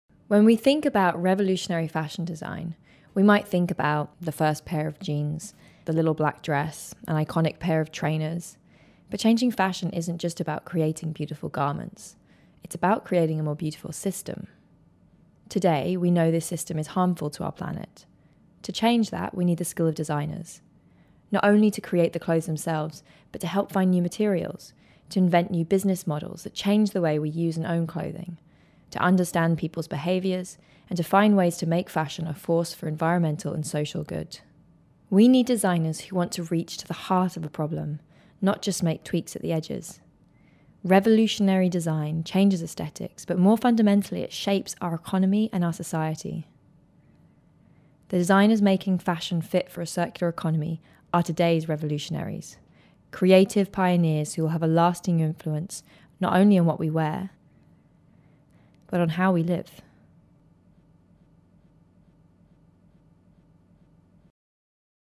Fashioning A Circular Future | RSA Student Design Awards Winner | Moving Pictures
Animator: Hannah Jesse
Voice: Lily Cole

BRIEF: Moving Pictures, conceive and produce an animation to accompany one of two selected audio files that will clarify, energise and illuminate the content